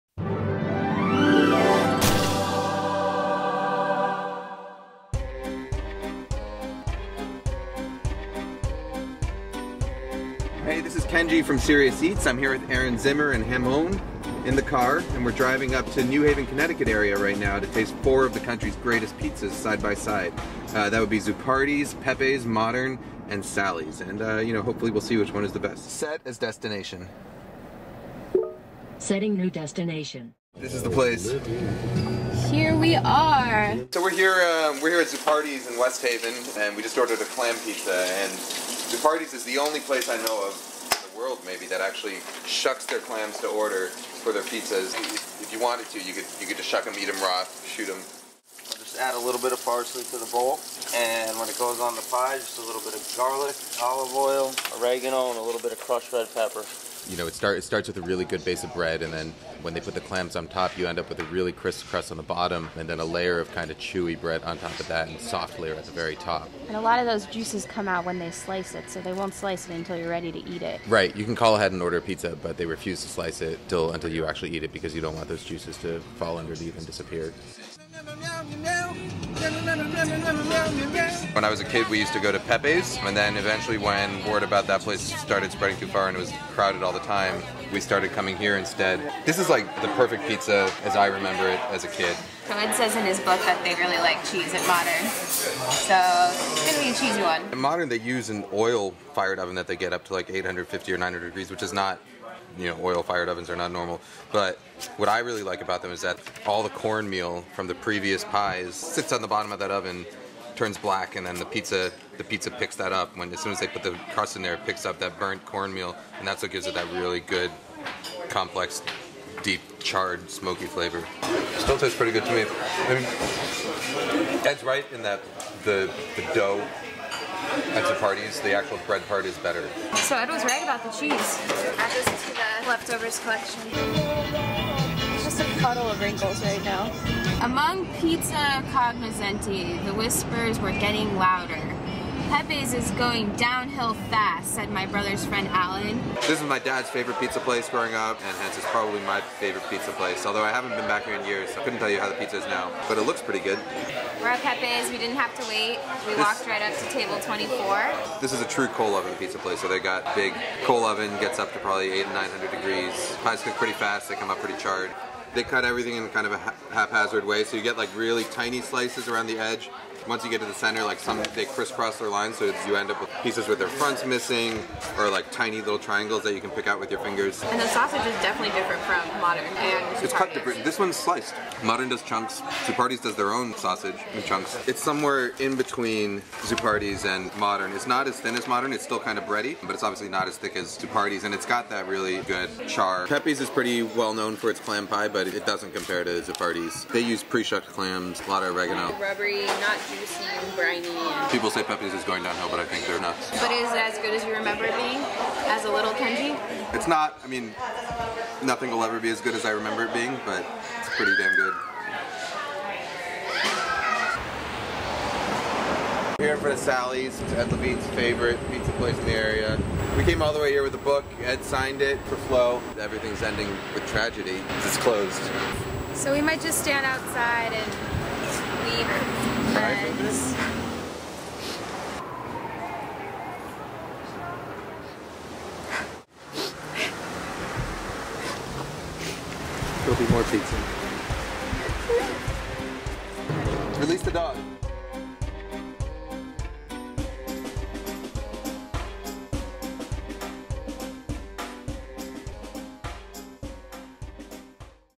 Serious Eats: A Pizza Road Trip in New Haven, CT
It's time for an epic pizza road trip with SE editors Kenji and Erin (and Hambone, too!). Destination: New Haven, CT.